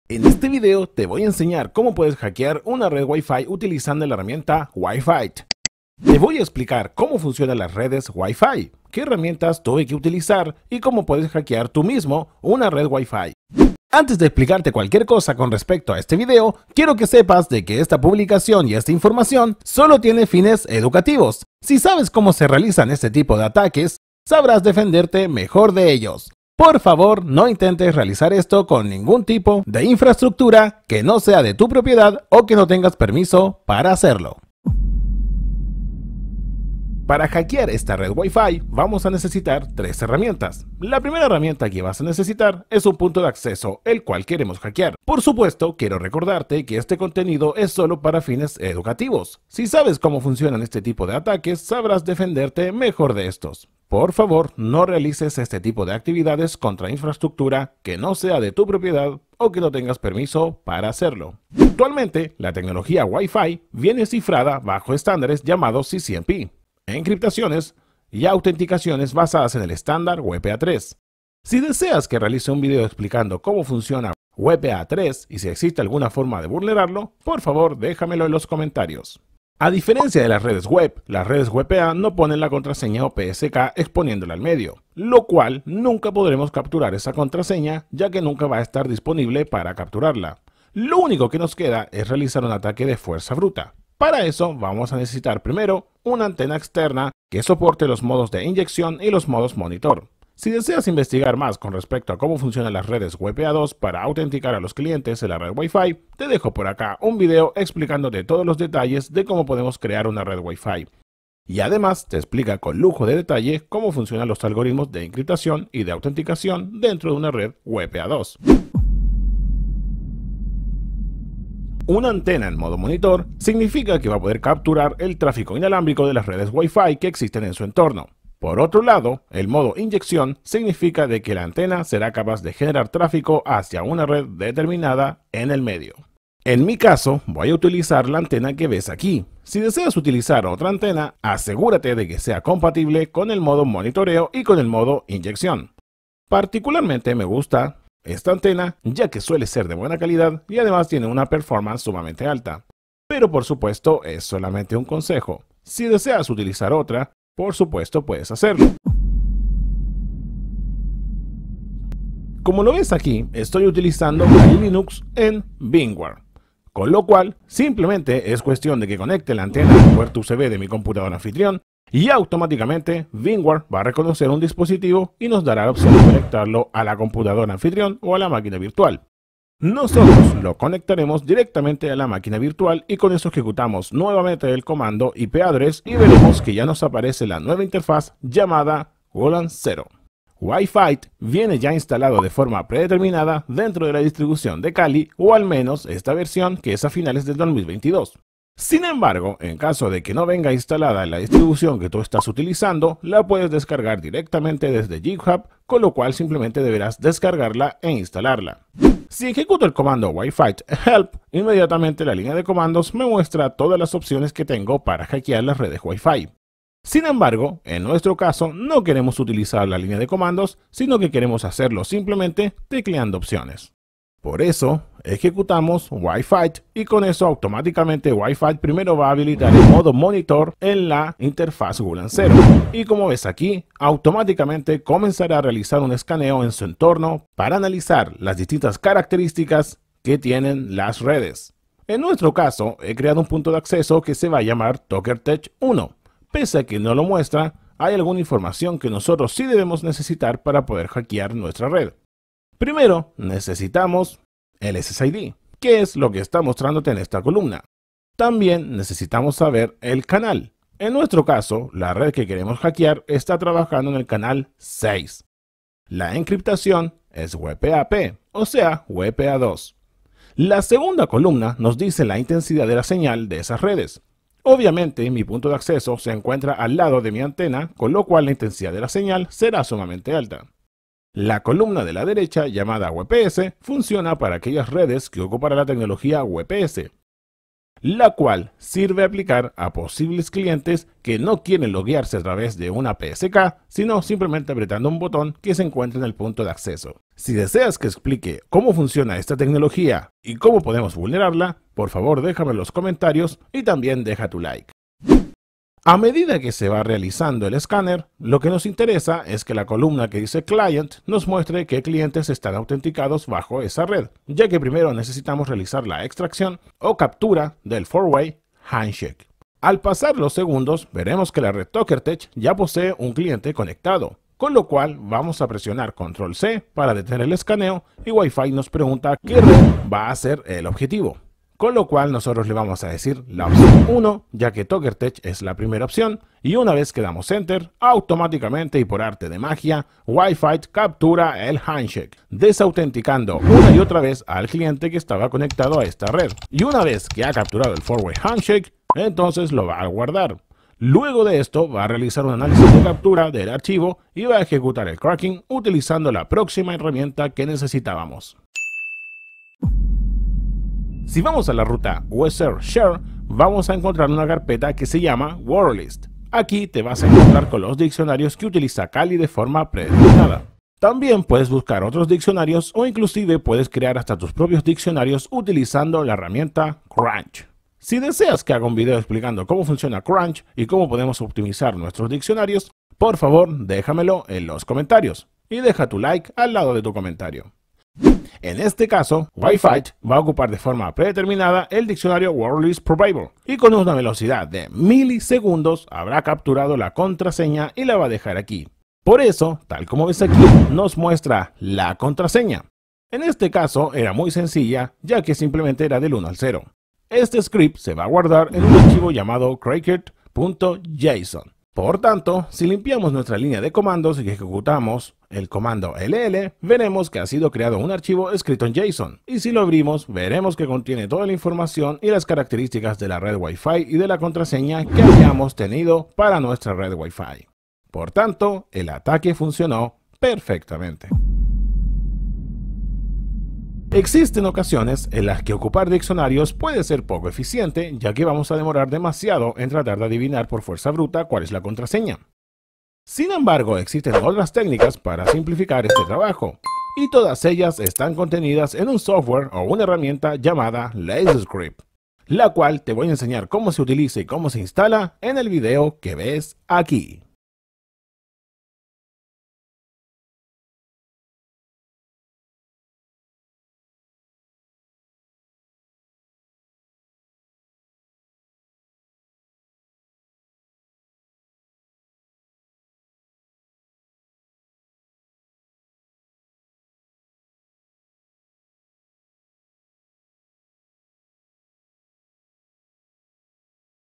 APRENDE WIFI HACKING de Forma Ética y Legal - ASI LOS HACKERS ACCEDEN A TU WIFI | Y ASI TE PROTEGES
Antes de explicarte cualquier cosa con respecto a este vídeo quiero que sepas de que esta publicación y esta información sólo tiene fines educativos. Si sabes cómo se realizan este tipo de ataques sabrás defenderte mejor de ellos. Por favor no intentes realizar esto con ningún tipo de infraestructura que no sea de tu propiedad o que no tengas permiso para hacerlo.

Soy dueño de todo el equipo usado para esta demostración. No se produjo ningún ataque real en ningúna infraestrucutura. Solo usa las herramientas demostradas en este video en redes que tengas permiso para atacar. Utiliza las herramientas de forma ética para mejorar la seguridad de la red.

 / tokertesh